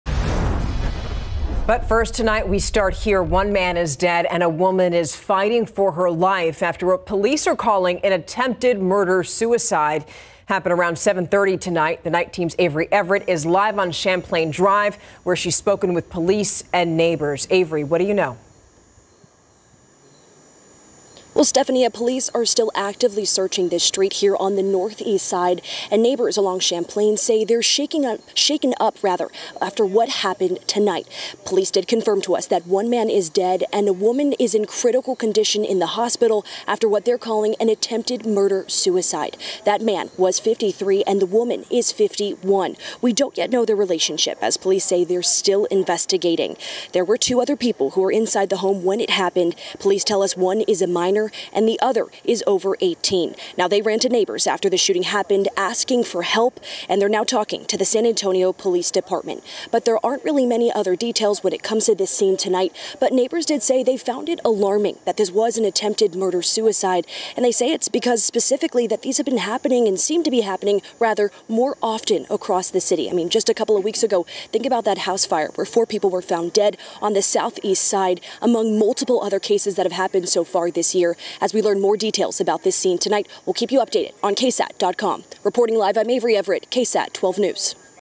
Man dead, woman in critical condition after attempted murder-suicide on Northeast Side, SAPD says
San Antonio police said a man was found dead, and a woman was rushed to a hospital after an attempted murder-suicide shooting Thursday night.